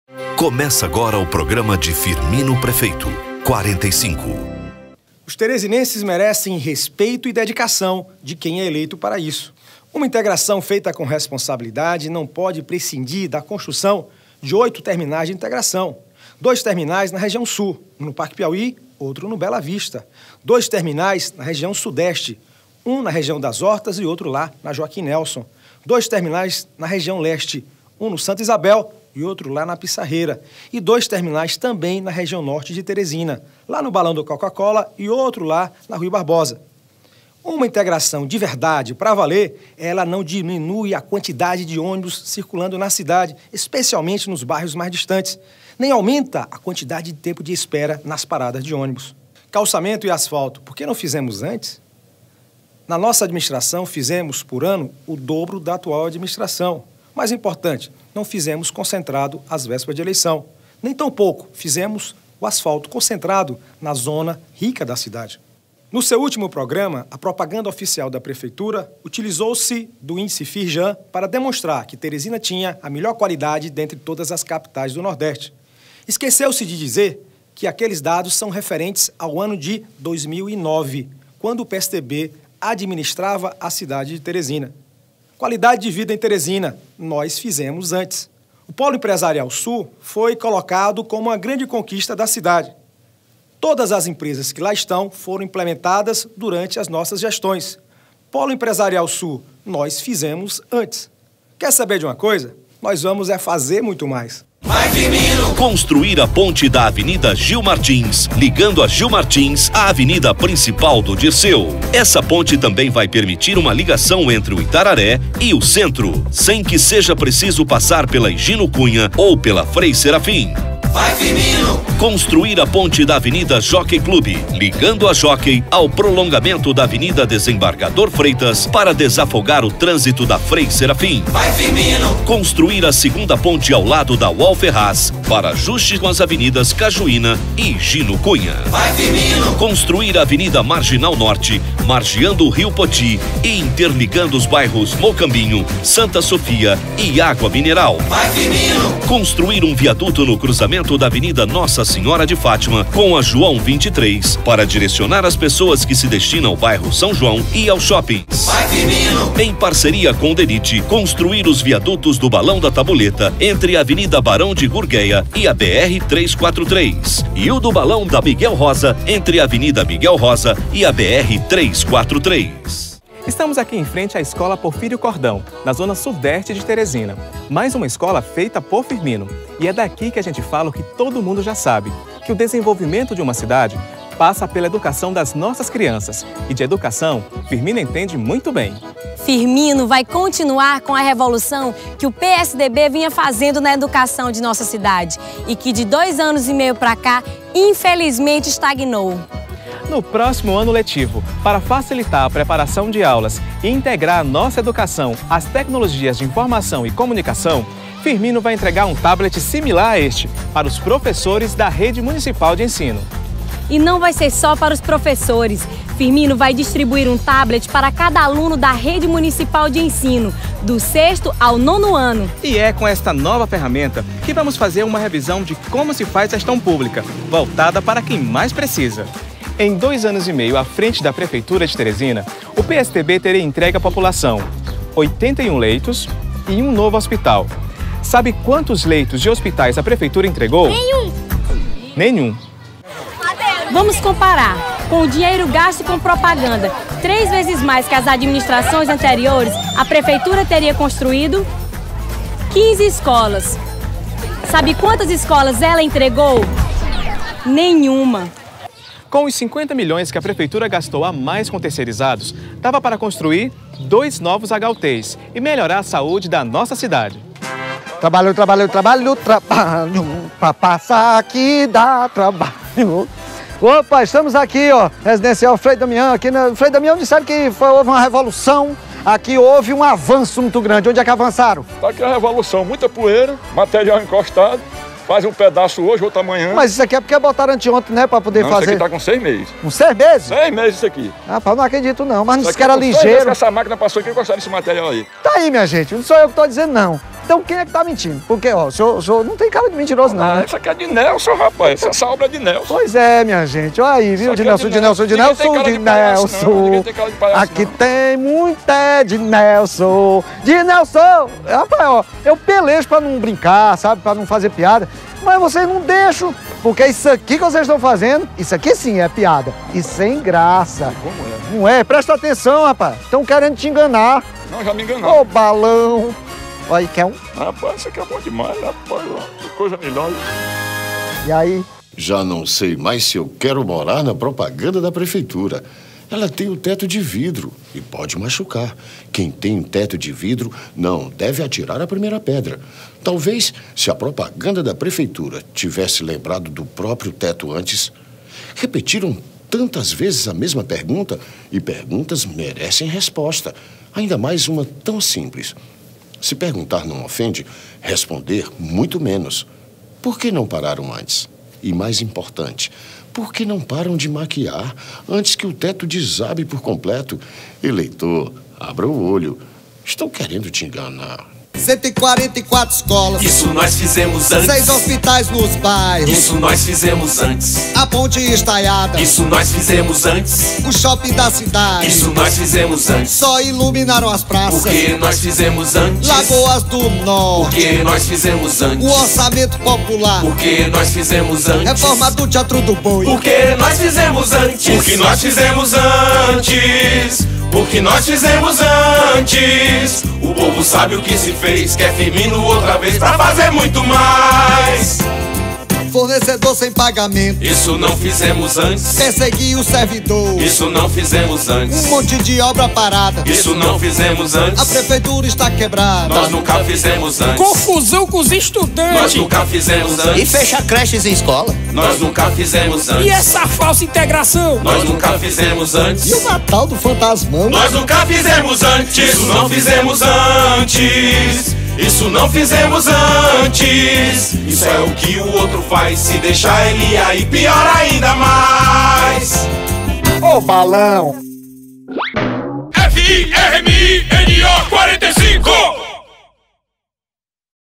Firmino 45 - Programa 16 - Teresina sabe o que fizemos
Continua a aula de gestão pública.

Neste programa, conheça um pouco mais sobre o programa de inclusão digital que distribuirá tablets a alunos e professores da rede municipal de ensino. Além disso, Firmino explica alguns dados sobre o ritmo de produção da Prefeitura, com base em dados que podem ser encontrados nas edições da Prestação de Contas, publicadas anualmente e enviadas à população durante as gestões do PSDB.

Veja e tire suas conclusões.